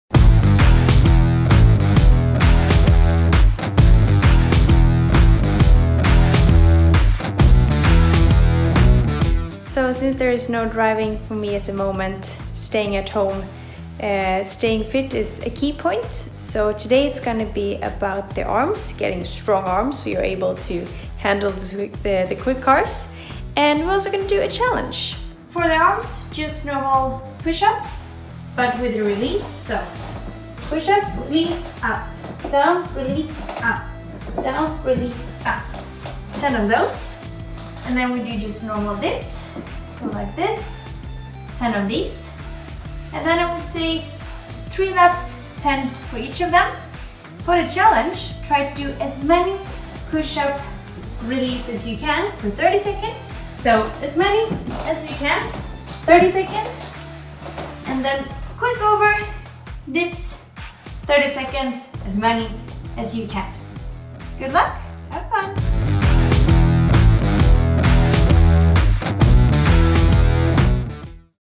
Extreme E - Continental - Mikaela@home - Episodio 3
Con el Covid pasamos tiempo confinados en casa. Mikaela nos da algunos ejercicios para mantenerse en forma.